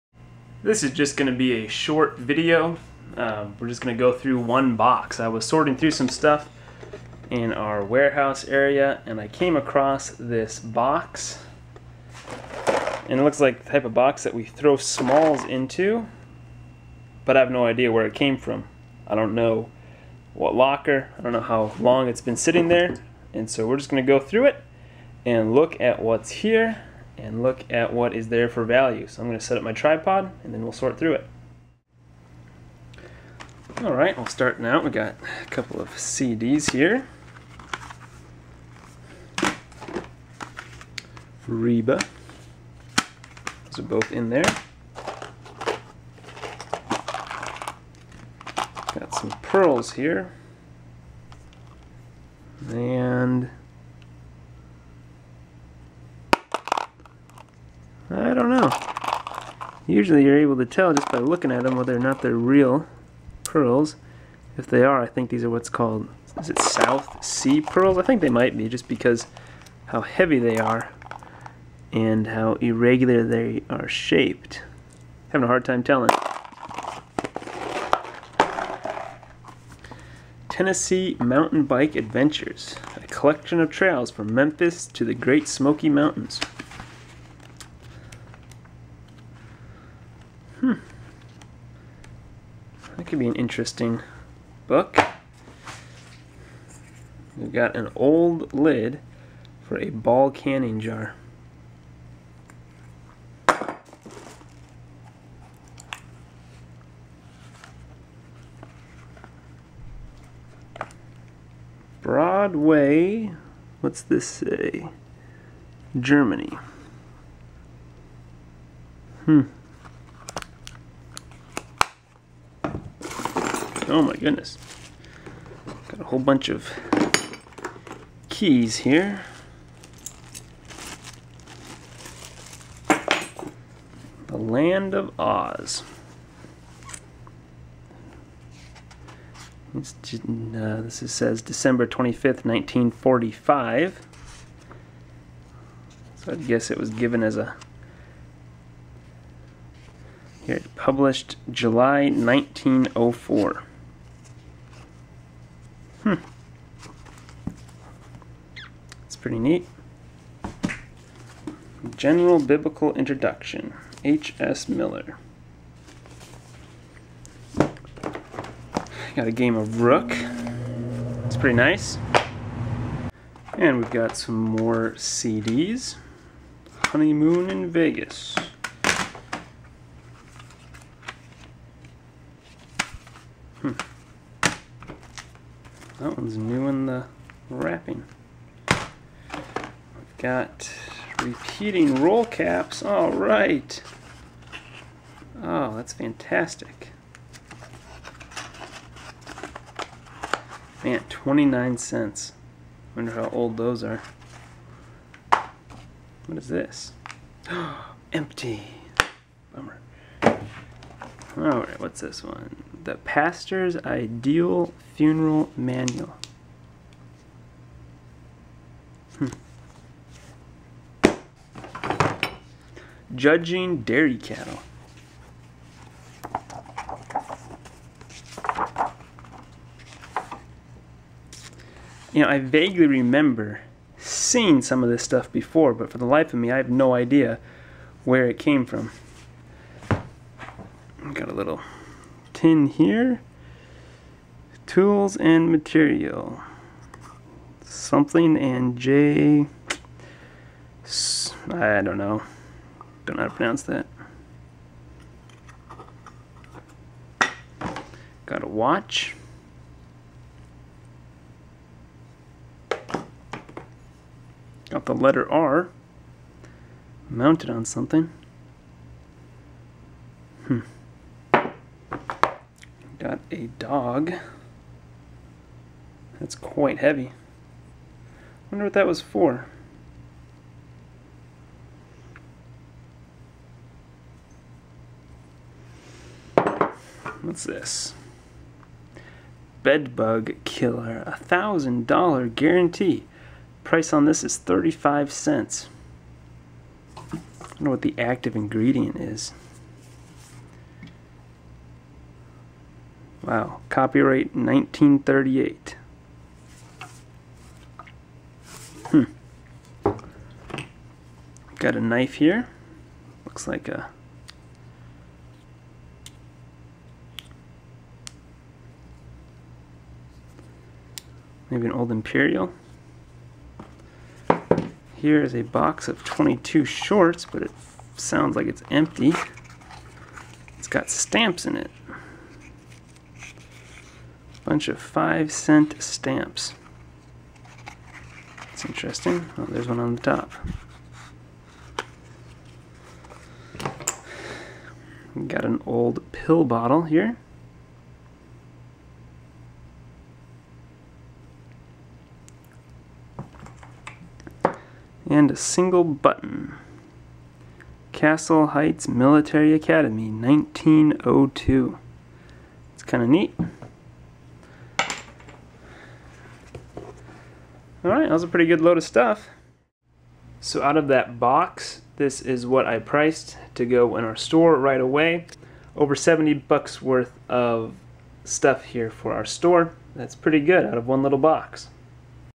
Mystery Box: Where Did it Come From and What's Inside? Great Smalls out of a Storage Locker!
I found this box back in our warehouse. I don't know which locker it came from or how long it has been sitting back there, but there were some good items inside!

We spend tens of thousands of dollars every year on storage locker auctions. Every day we go to work to answer the question: What's in the next box?